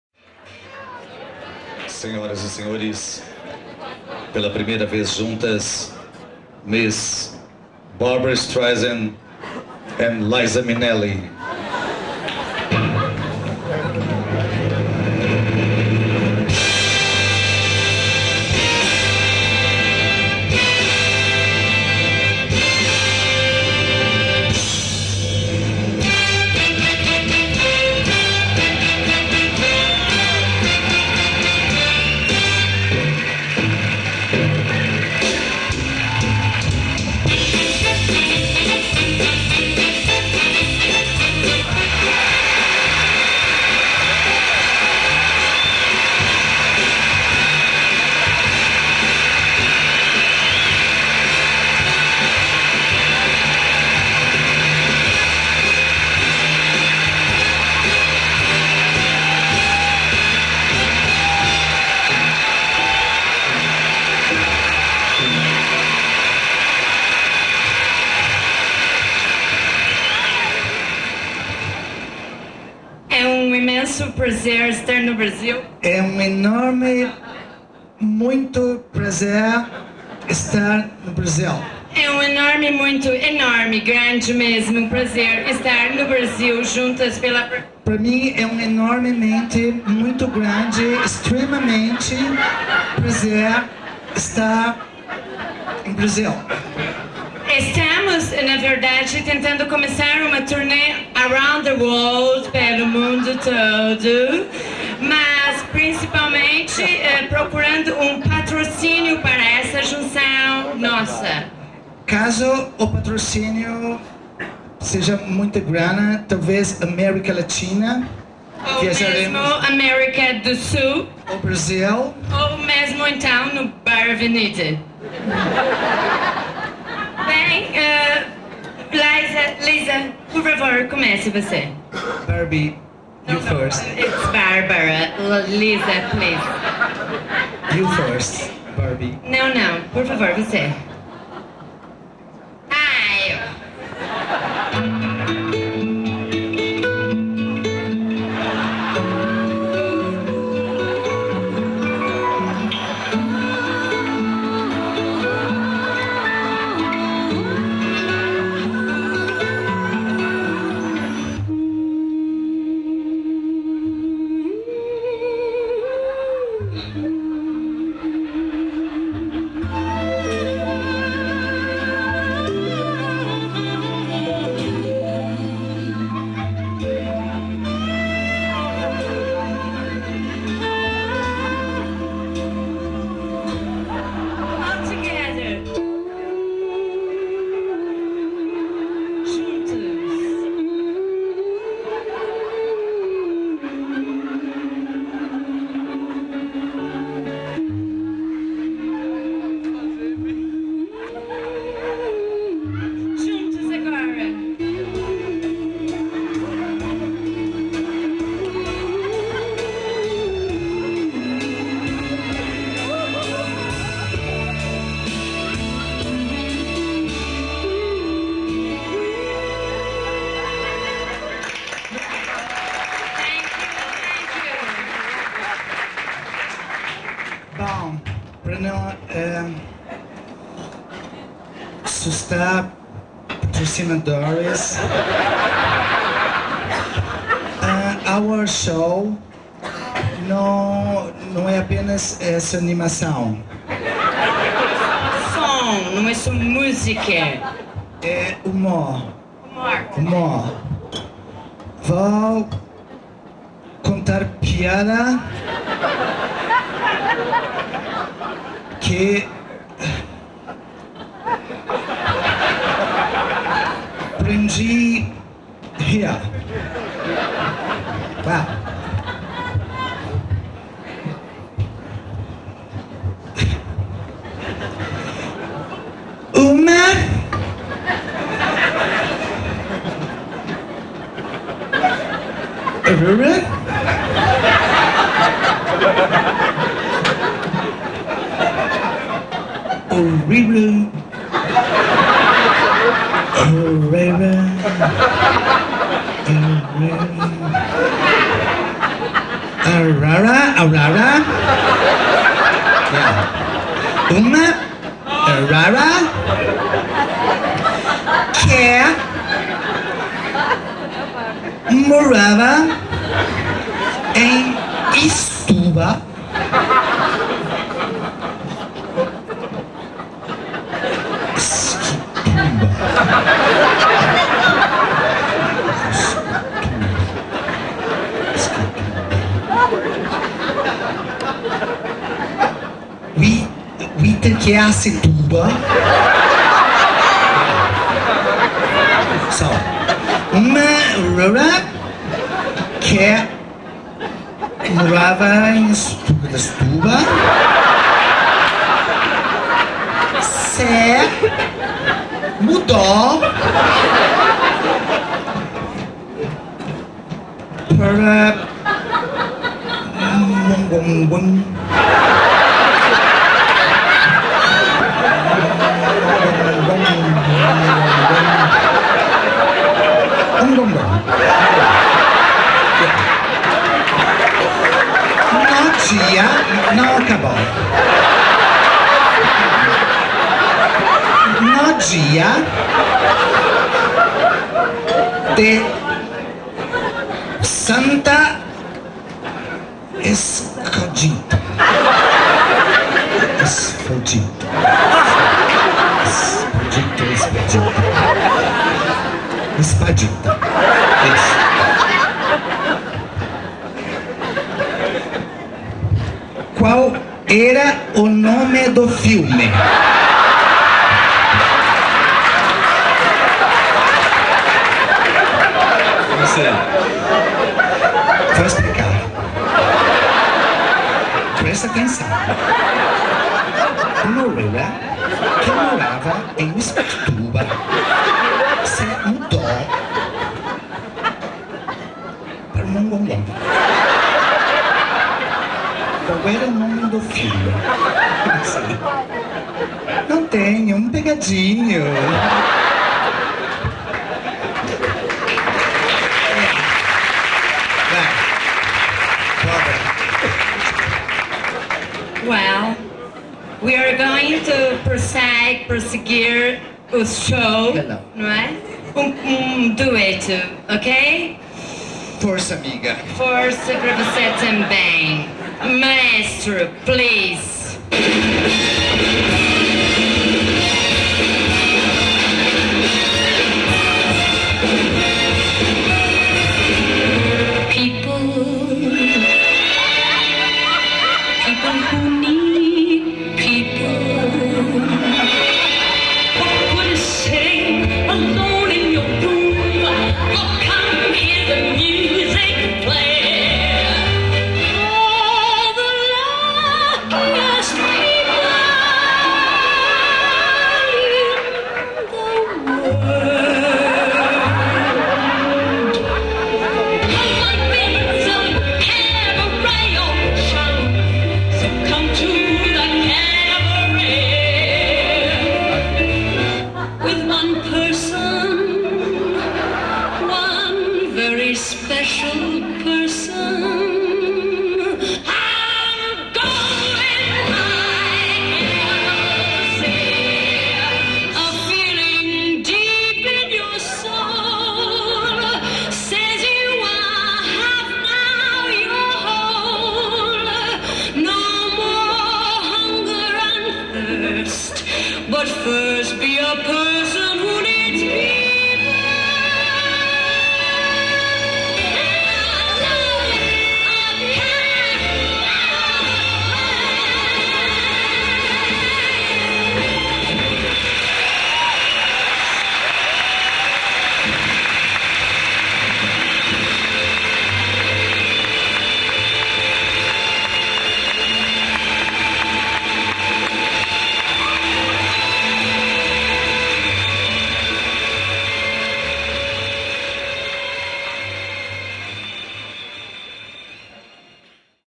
Ilana Kaplan & Octavio Mendes - Barbara Streisand e Liza Minelli
Barbara Streisand e Liza Minelli juntas pela primeira vez no Brasil no Bar Avenida  Terça Insana.

Esquete e personagens registrados, autoria dos próprios atores.
 (Direito de exibição: Daniel De Rogatis - autorizada por Ilana Kaplan e Octavio Mendes).